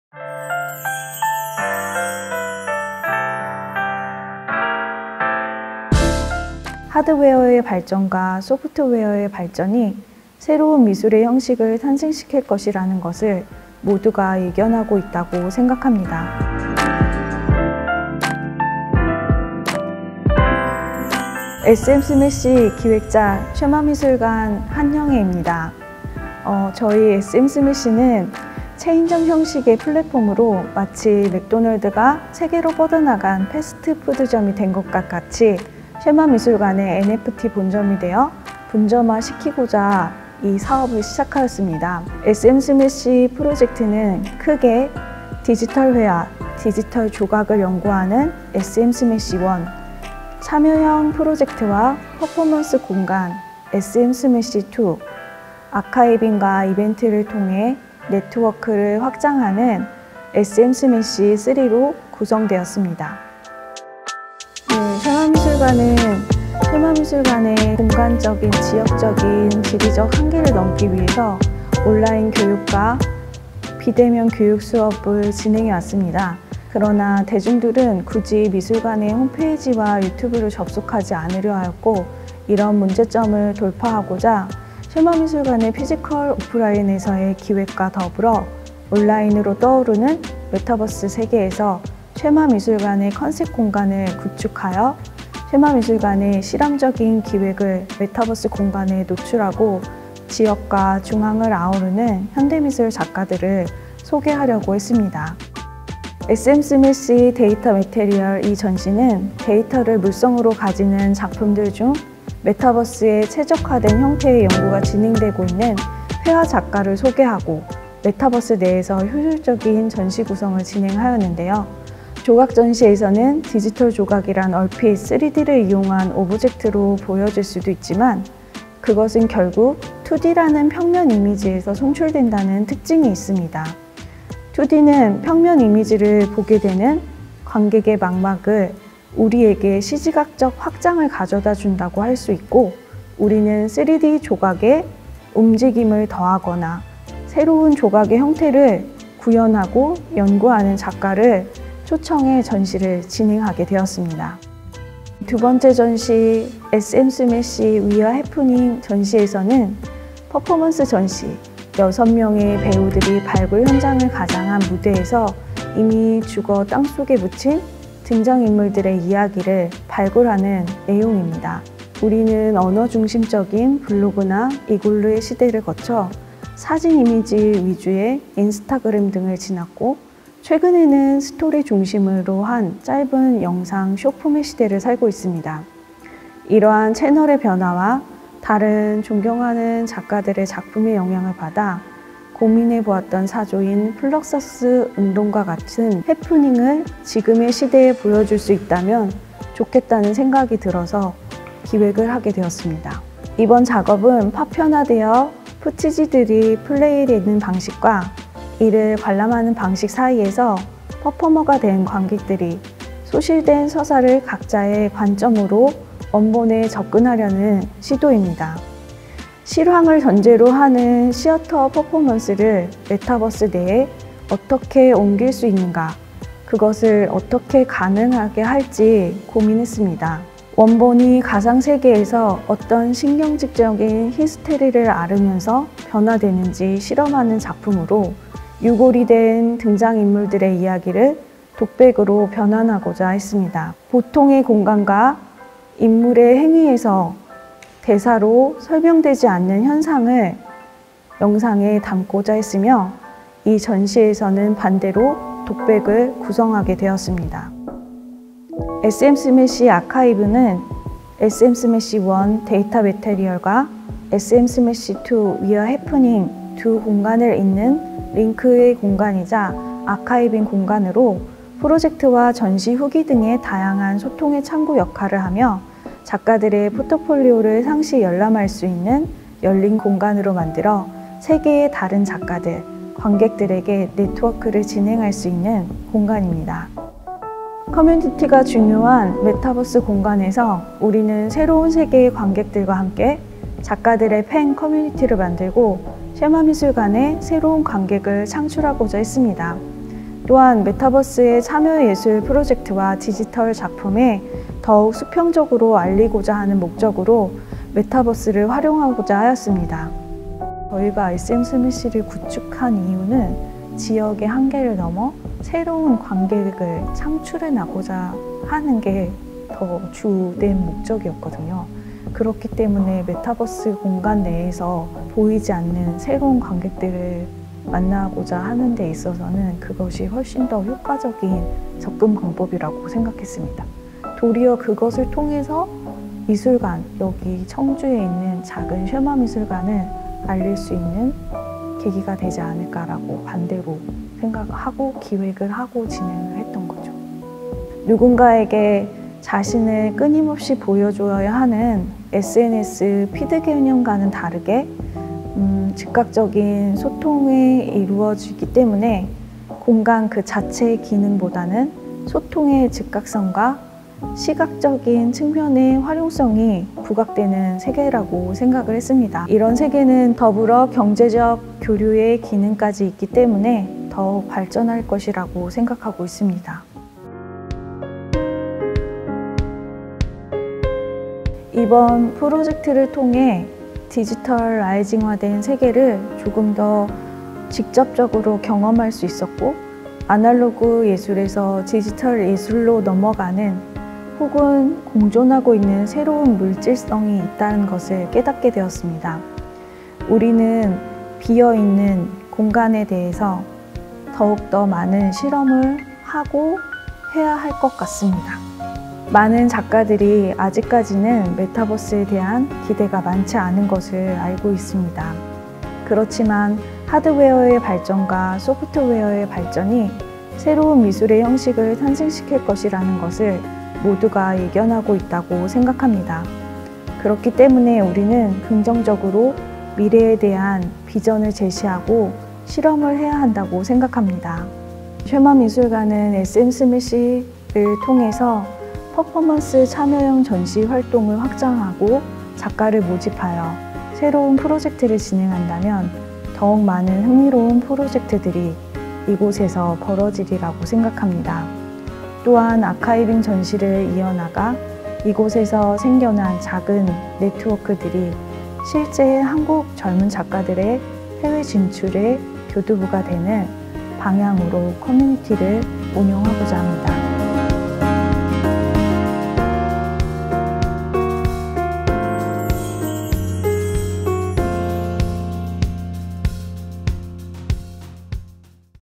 [메타버스 예술｜선정자 인터뷰] 쉐마미술관 - SM Smash｜2022년 메타버스 예술활동 지원사업
한국문화예술위원회는 문화체육관광부와 함께 ‘메타버스 예술활동 지원사업’을 통해 확장가상세계(메타버스)에서 새로운 예술 창작에 도전하는 예술가분들을 지원합니다.

쉐마미술관은 지역미술관의 한계를 벗어나고 새롭게 맞이하는 디지털 플랫폼에서의 활발한 전시기획 활동과 한국작가들의 해외진출을 돕고자 메타버스 플랫폼 Decentraland 에 분점 SM-Smash를 만들었습니다.